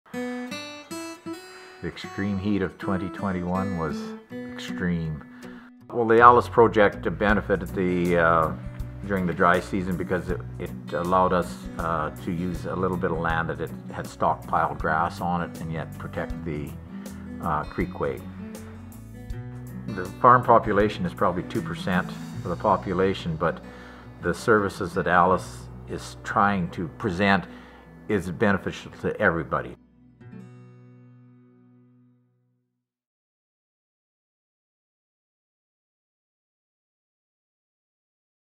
Allan Goddard, ALUS Brazeau
Allan Goddard manages roughly 450 acres for his cow-calf operation on his 800-acre property east of Drayton Valley in Alberta. With support from ALUS Brazeau, Allan has introduced exclusion fencing along a creek, adding to the natural function and long-term resilience of this unique ecosystem. Allan’s projects provide ecosystem goods and services for the present, as well as future generations. ALUS is proud to work farmers on watershed health throughout Canada.

Located in west-central Alberta, Brazeau County launched its ALUS program in the spring of 2016 and continues to gain momentum in the community. ALUS Brazeau, through its coordinators, participants and leaders, has worked hard to improve riparian habitat, increasing flood and drought resilience and improving water quality within the Modeste subwatershed.